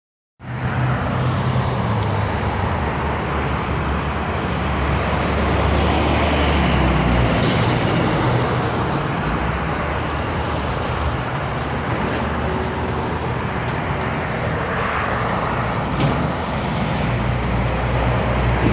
Pontiac G8 GT Blocked by Dodge Neon 20161014 142438
By 2:24 PM this Pontiac G8 GT drives up. Only one thing is wrong, a Dodge Neon takes up the right lane at the same time. Blocking the side profile when the Pontiac goes by. Same thing happened last yr w this car.